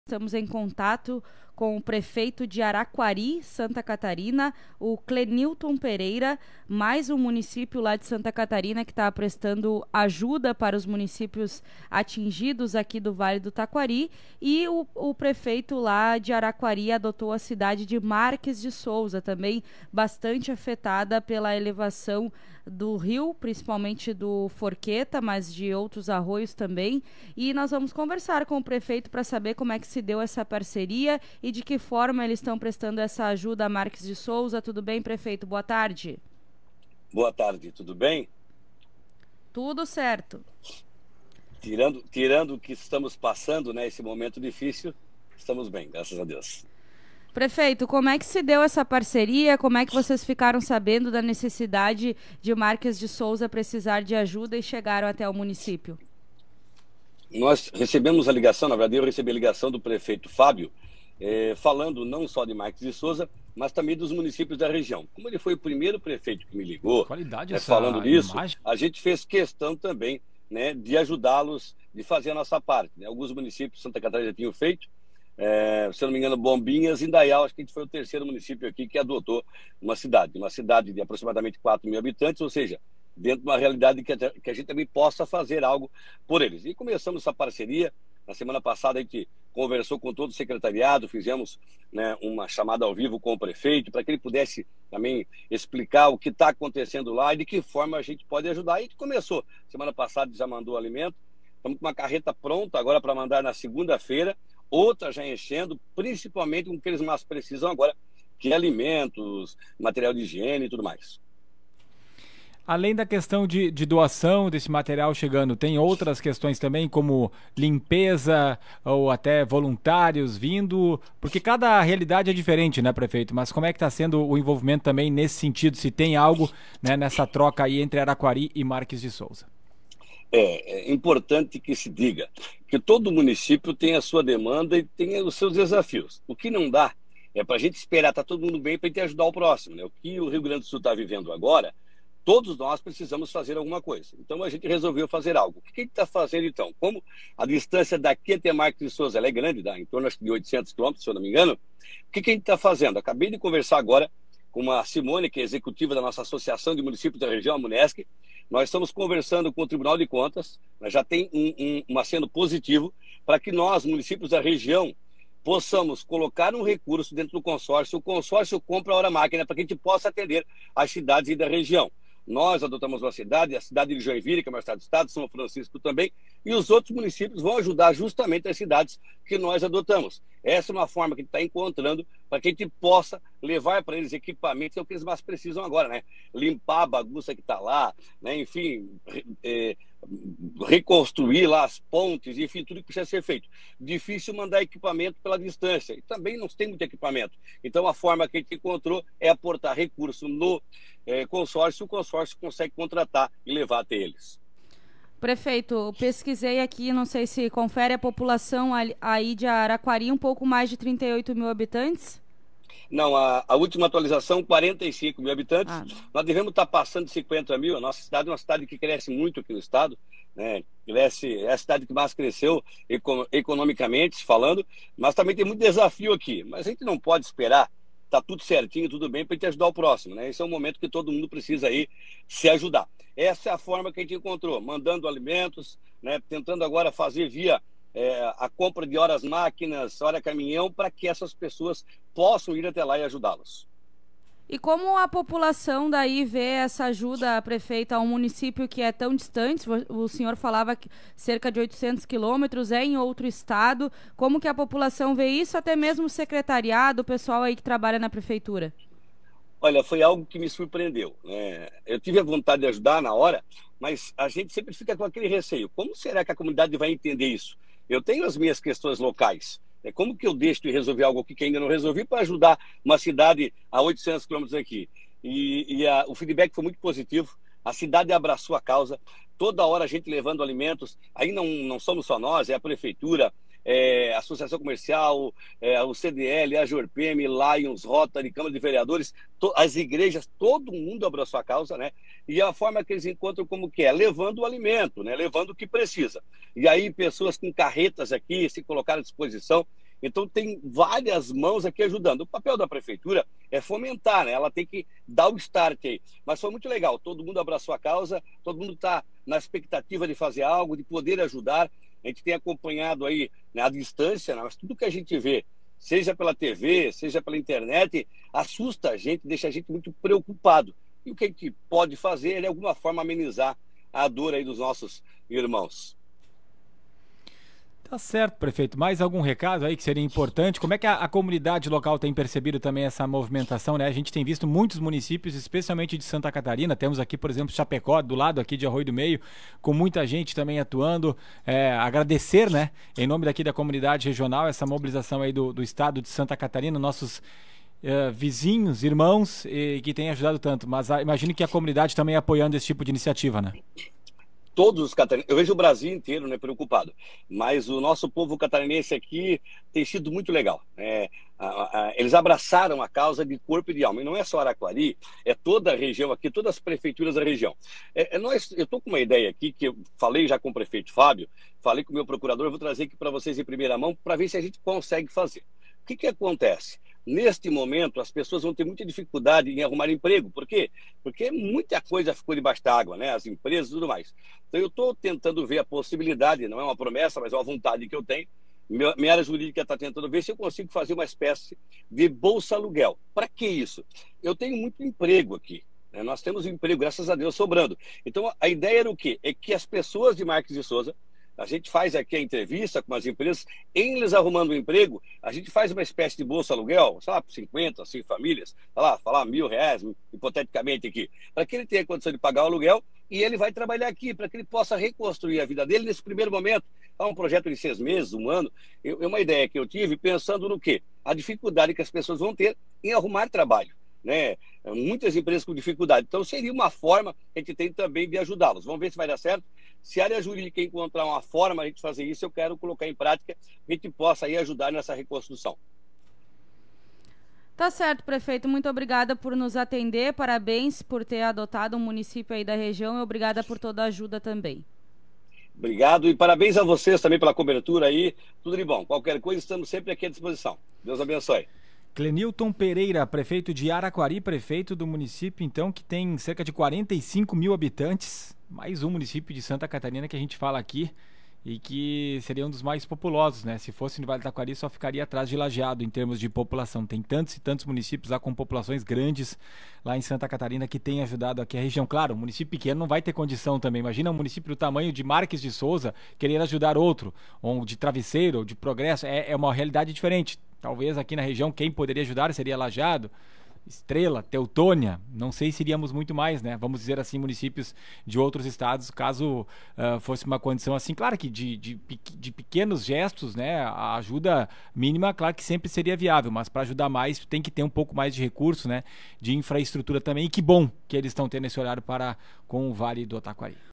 Araquari pensa em criar bolsa-aluguel para moradores de Marques de Souza trabalharem na cidade
O município catarinense “adotou” Marques e auxilia a cidade com doações e maquinário após a enchente